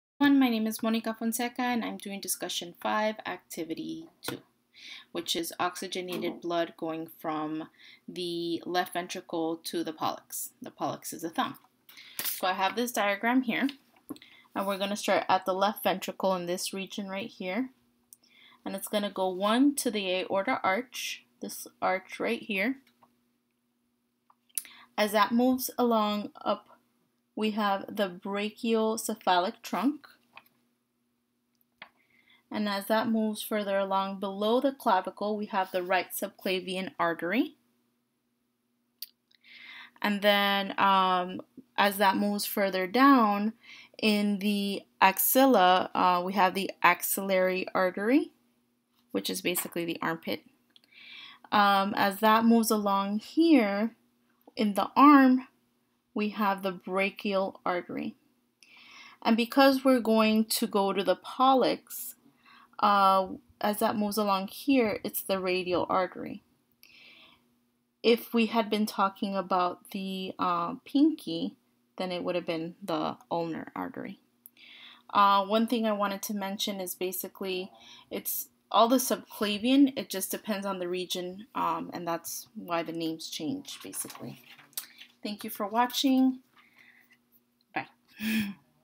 Discussion 5
Oxygenated blood from left ventricle to the pollex.